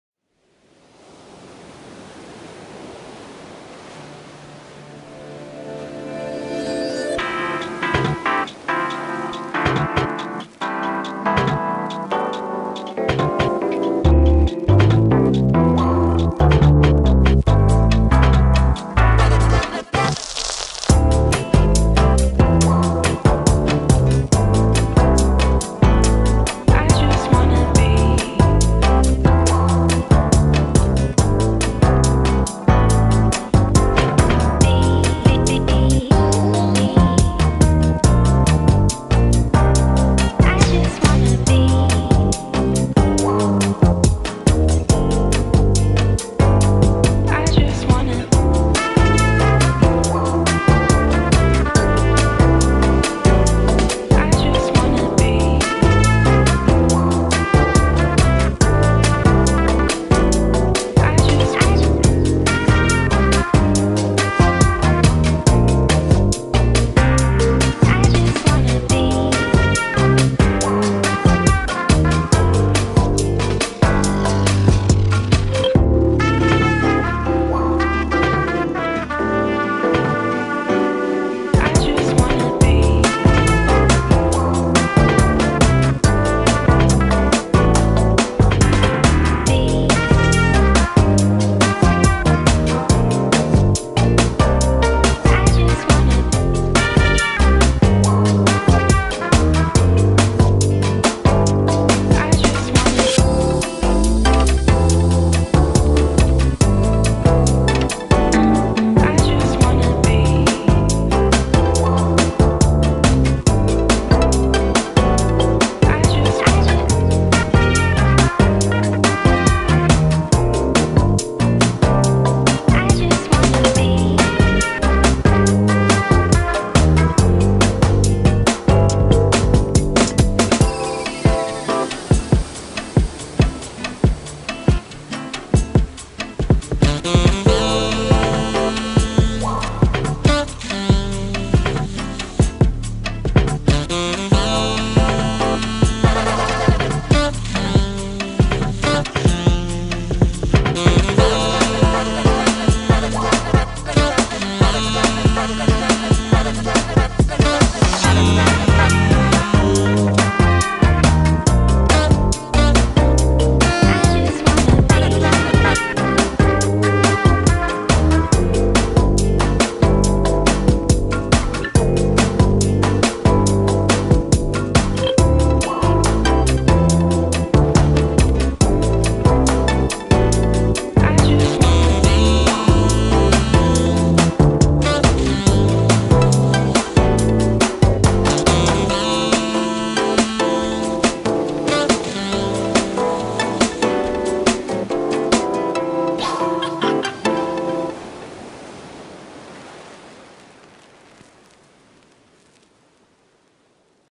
Chill Tropical Bossa Nova Vibe
Relaxing Tropical Bossa Nova Chill Out music.

Background: Beach in Sentosa, Singapore by Christian Chen (Unsplash.com)